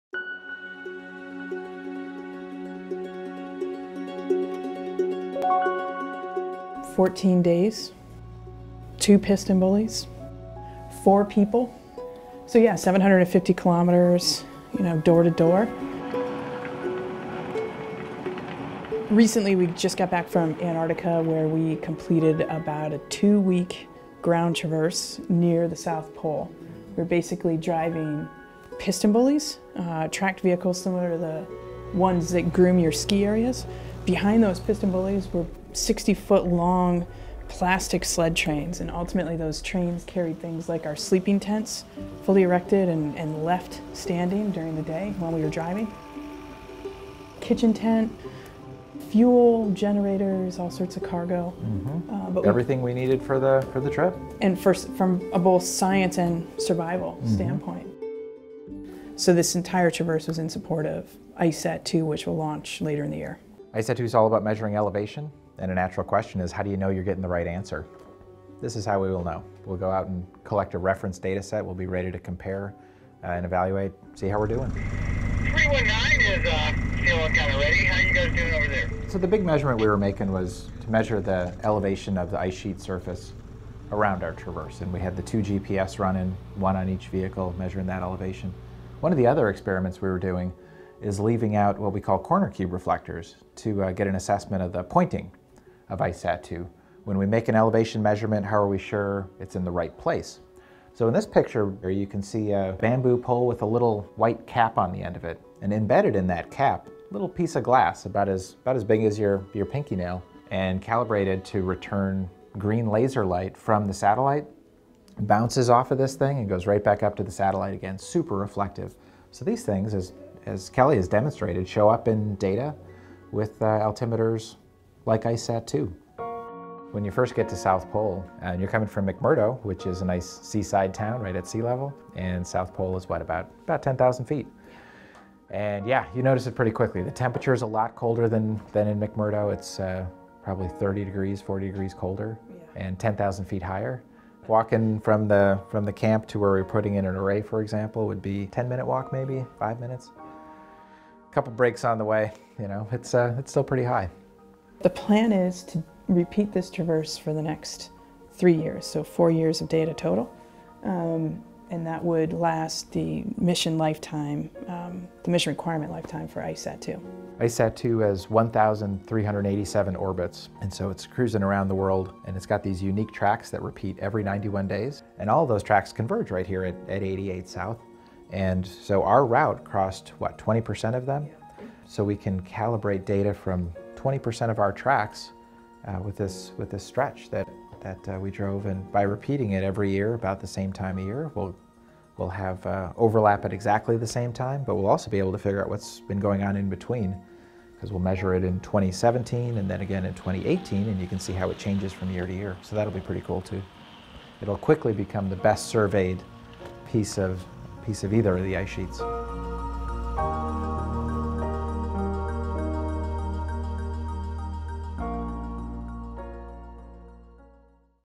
The 88-South Antarctic Traverse
With extreme cold-weather gear, scientific instruments, and two tank-like snow machines called PistenBullys, NASA scientists began a traverse on December 21 along section of the 88-degree south latitude line in an arc around the South Pole, documenting a new route across the Antarctic ice.

The 470-mile expedition in one of the most barren landscapes on Earth provided the best assessment of the accuracy of data collected from space by the Ice Cloud and land Elevation Satellite-2 (ICESat-2), set to launch in 2018.

With a fast-firing laser instrument, ICESat-2 will measure the elevation of ice sheets and track how much they change over time. Even small amounts of melt across areas as vast as Greenland or Antarctica can result in large amounts of meltwater contributing to sea level rise.

To help document this, ICESat-2's height change measurements will have a precision of less than an inch – ground-truthed, in part, with efforts like this Antarctic campaign. The team will collected precise GPS data of the elevation at 88 degrees south, where ICESat-2’s orbits converge, providing thousands of points where the survey measurements can be compared to satellite data.

Credit:
NASA's Goddard Space Flight Center.